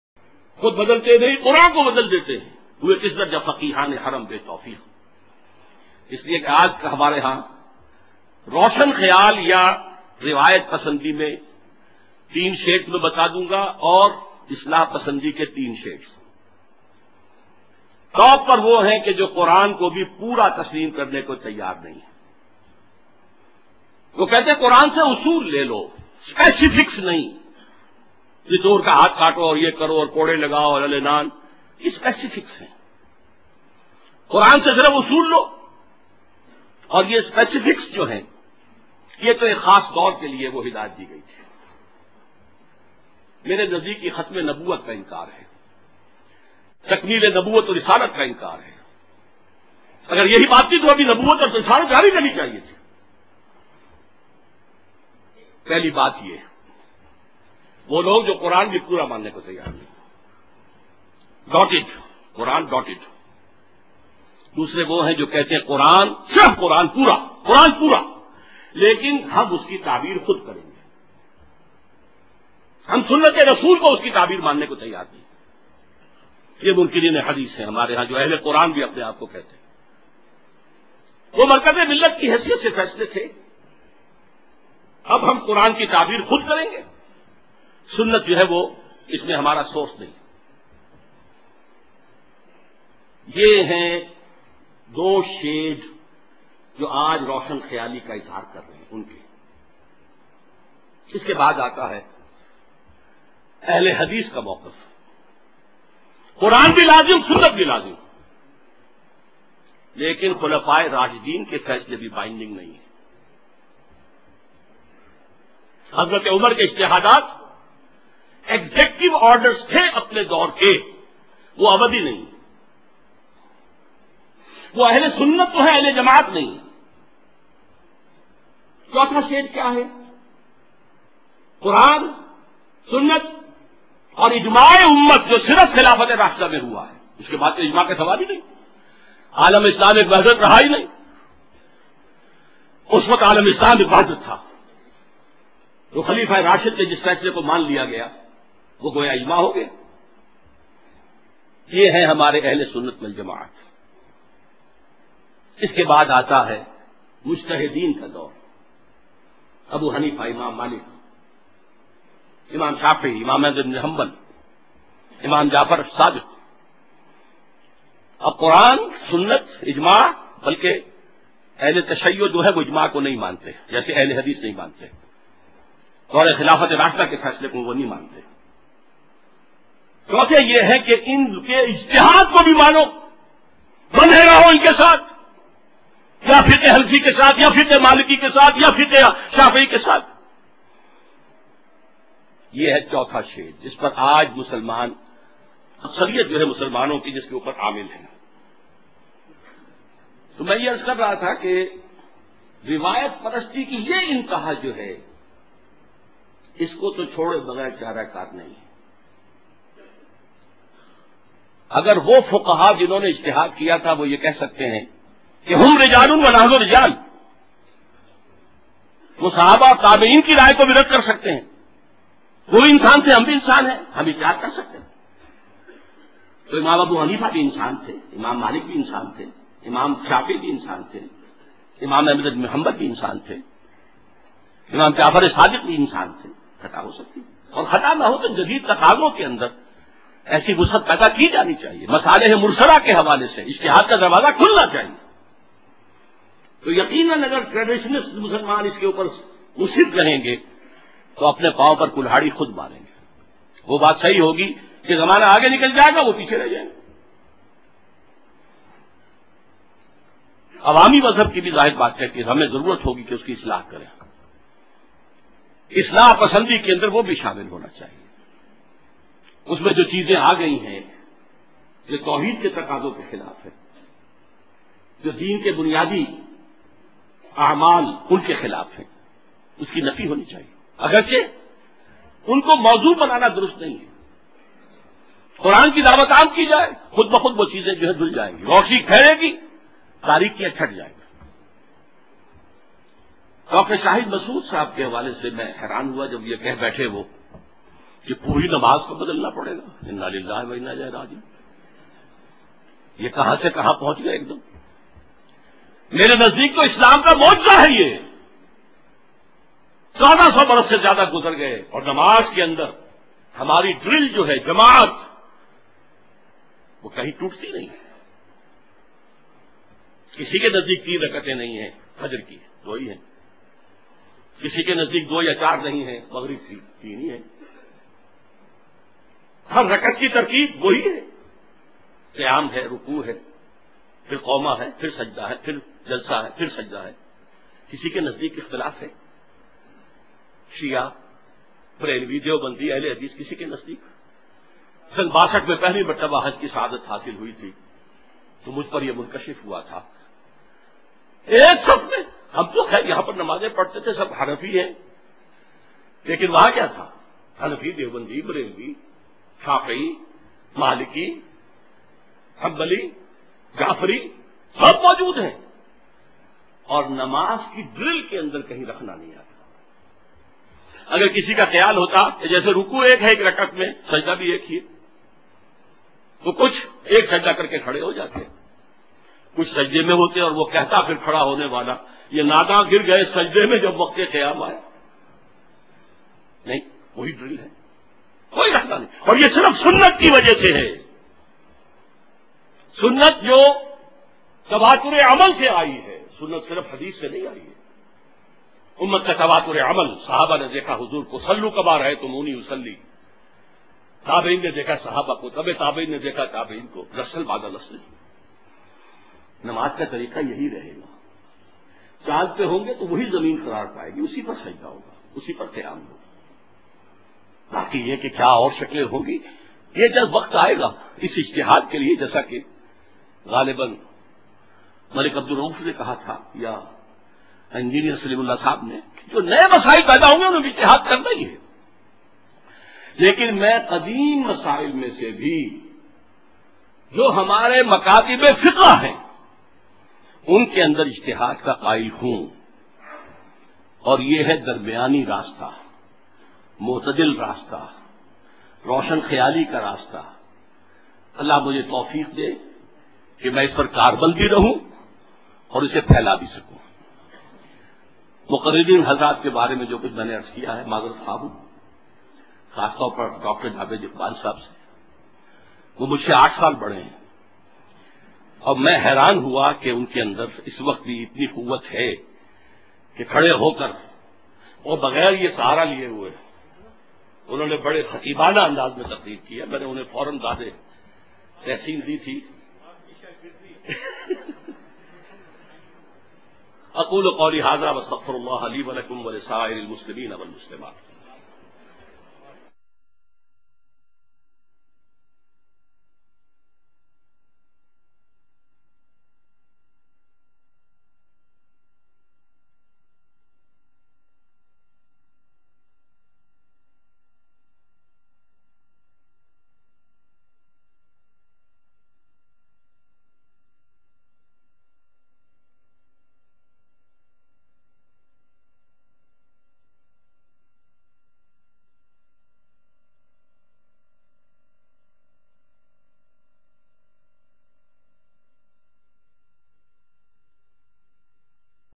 7/7- Western Enlightenment vs. Islamic Enlightenment - Ideological difference in Muslims
The real face of western enlighten moderation